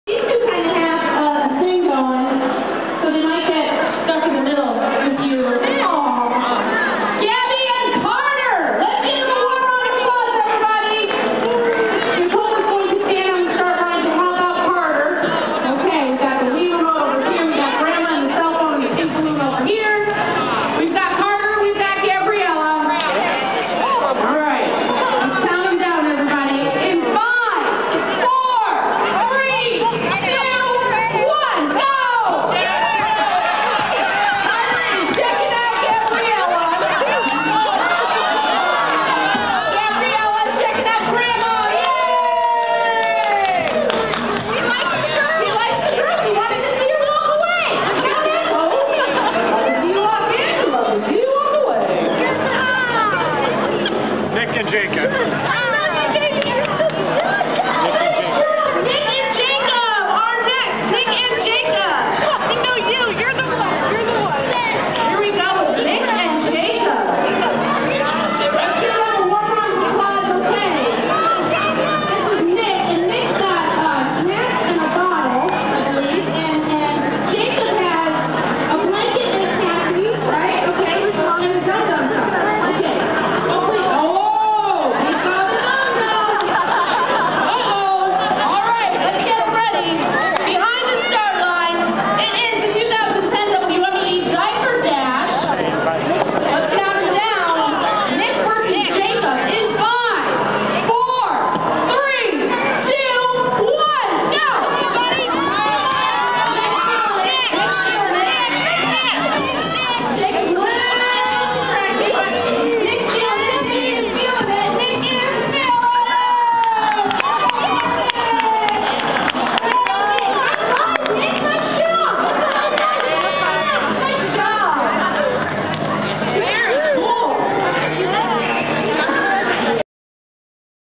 Diaper Dash Semi Finals - 97.3 WMEE Baby Fair & Family Expo
Semi final round for a year's worth of diapers from Meijer!